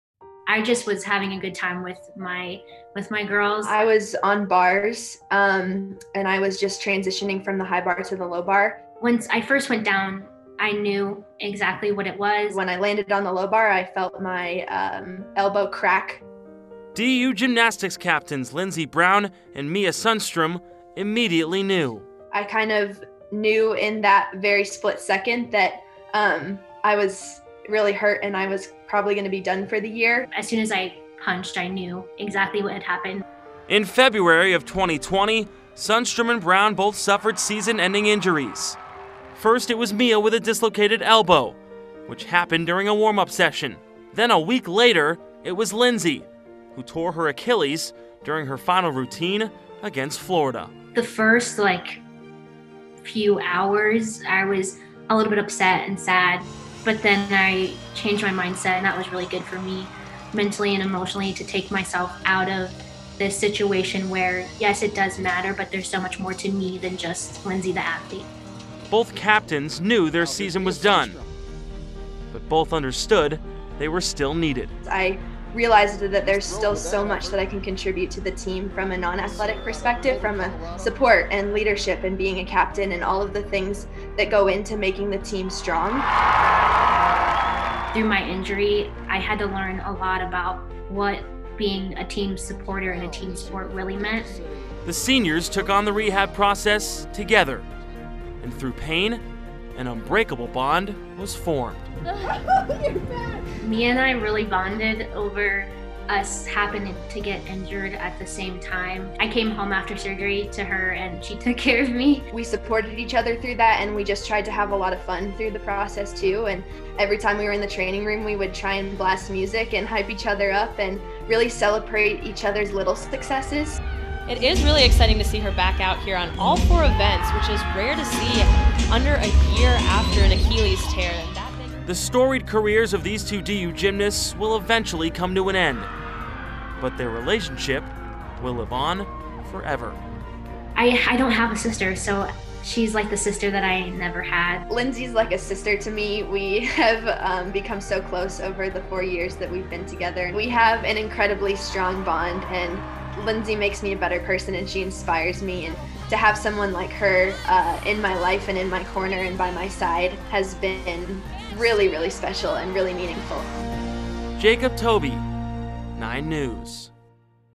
DU gymnasts create lifelong friendship while recovering from injuries
In February of 2020 Lynnzee Brown and Mia Sundstrom both suffered season-ending injuries, but they took on the rehab process together and formed an unbreakable bond.